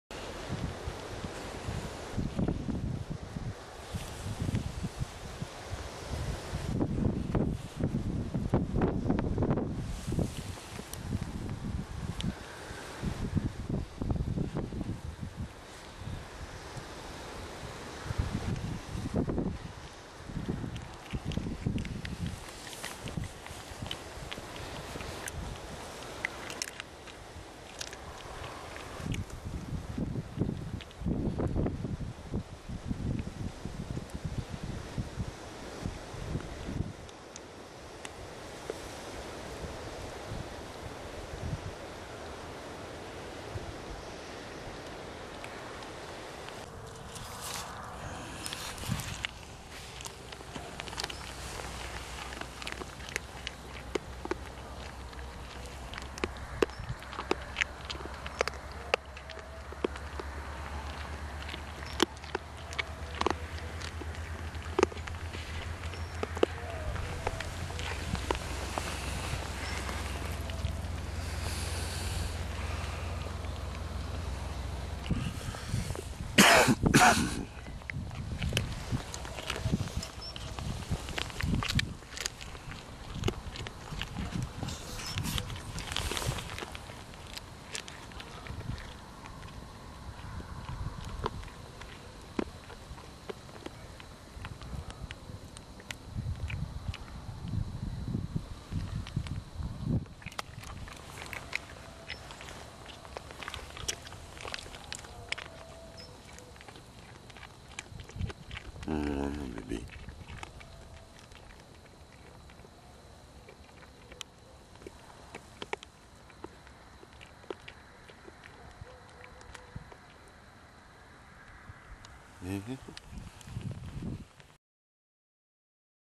Manger des glands.mpg
roe deer eating acorn of oaks